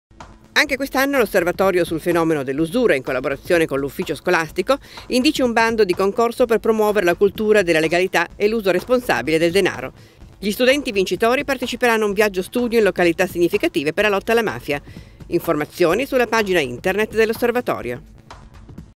Le false promesse del gioco d’azzardo
Anche quest’anno, l’Osservatorio sul fenomeno dell’usura, in collaborazione con l’Ufficio scolastico, indice un bando di concorso per promuovere la cultura della legalità e l’uso responsabile del denaro. Gli studenti vincitori parteciperanno a un viaggio studio in località significative per la lotta alla mafia. Informazioni sulla pagina Internet dell’Osservatorio.